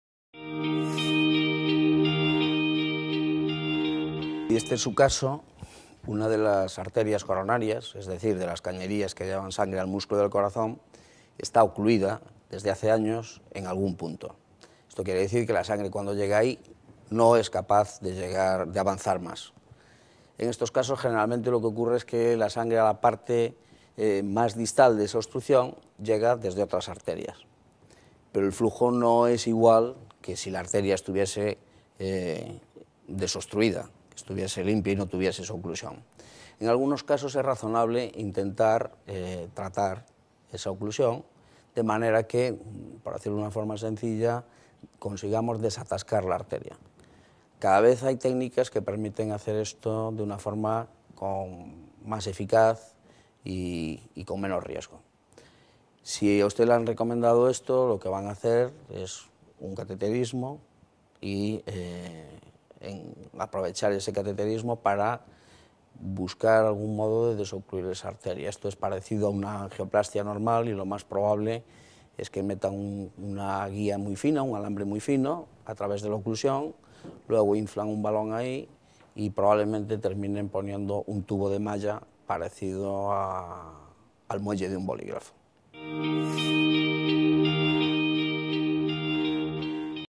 Tengo una arteria tapada hace años y ahora me han ofrecido abrirla, ¿qué me van a hacer?
Dr. José Manuel Vázquez (Complejo Hospitalario Universitario)